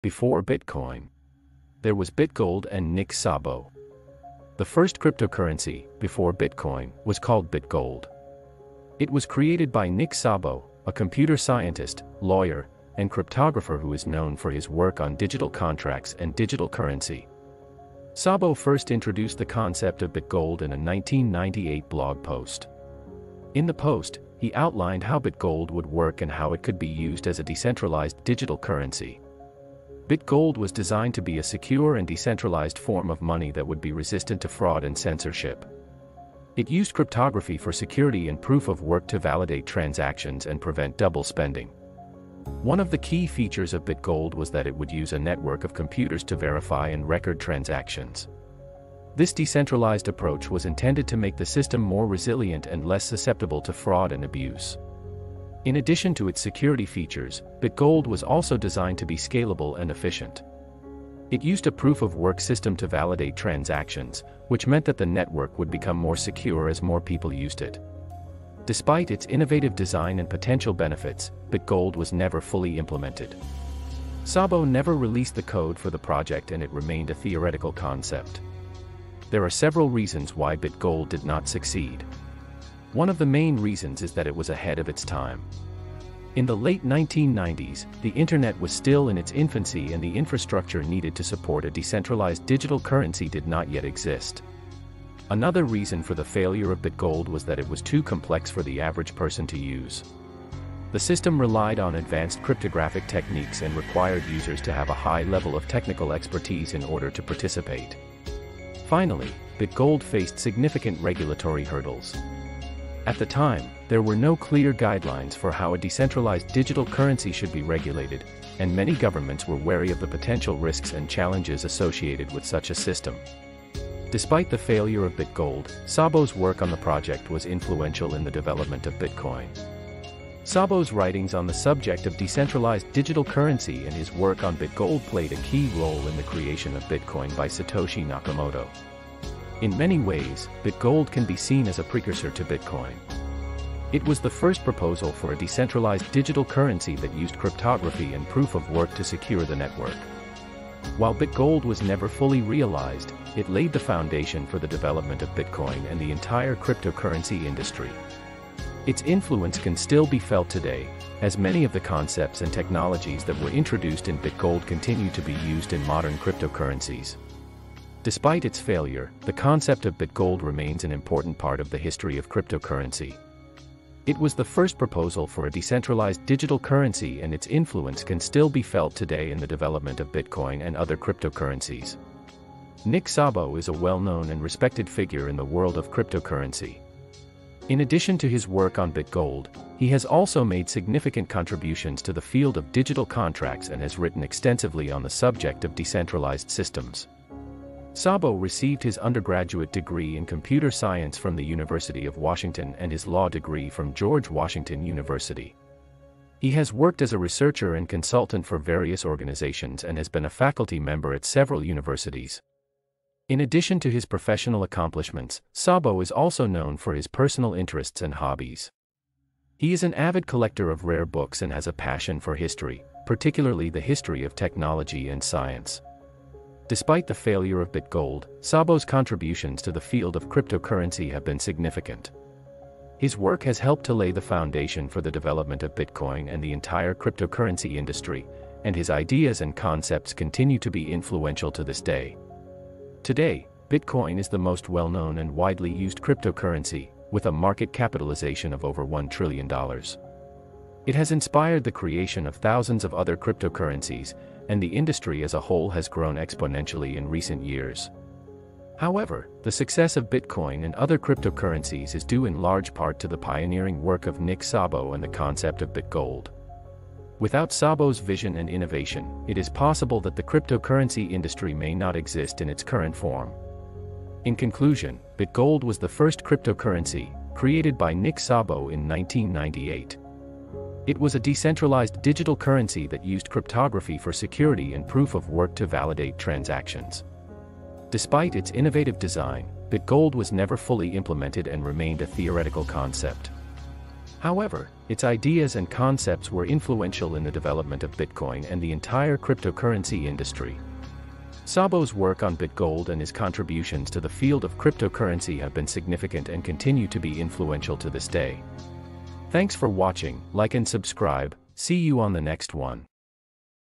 Before Bitcoin there was Bitgold and Nick Szabo
The first cryptocurrency, before bitcoin, was called Bit Gold. It was created by Nick Szabo, a computer scientist, lawyer, and cryptographer who is known for his work on digital contracts and digital currency.
Szabo first introduced the concept of Bit Gold in a 1998 blog post. In the post, he outlined how Bit Gold would work and how it could be used as a decentralized digital currency.